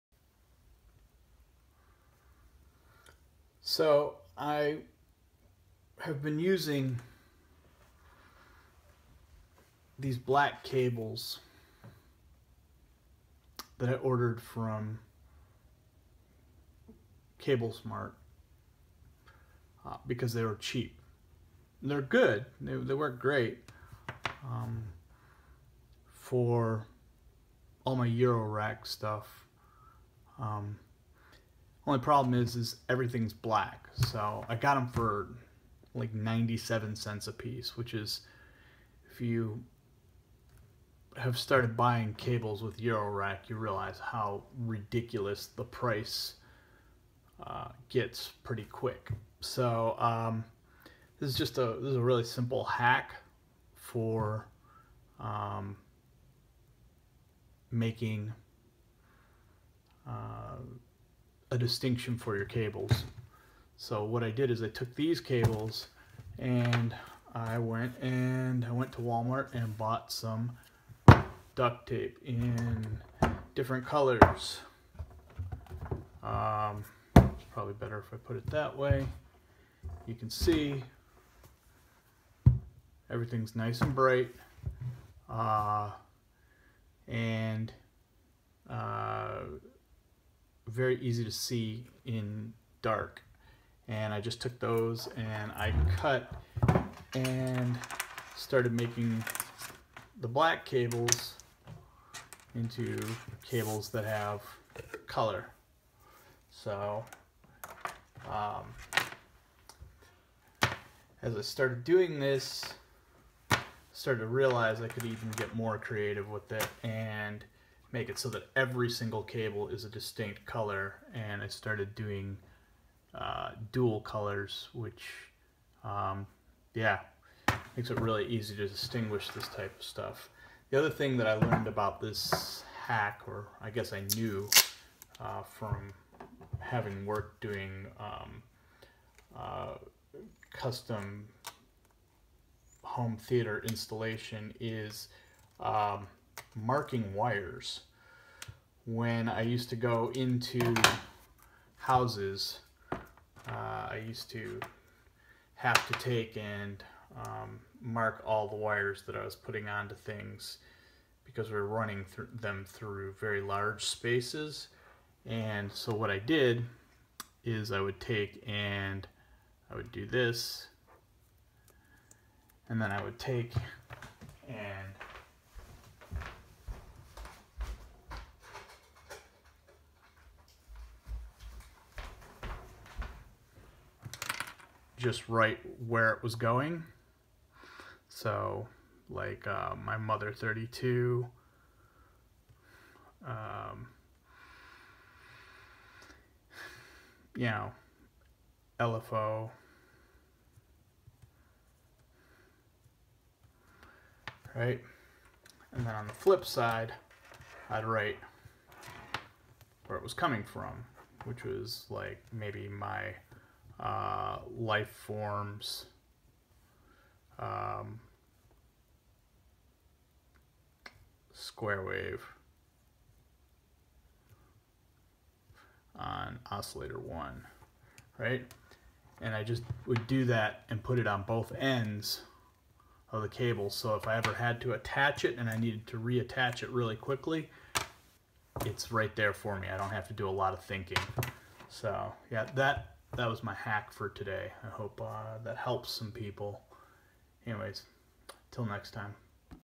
Hacks for Low Cost Eurorack Patch Cables
I came up with a simple yet effective hack for getting cheap, decent quality cables to battle the ridiculous cost of 3.5 mm eurorack cables.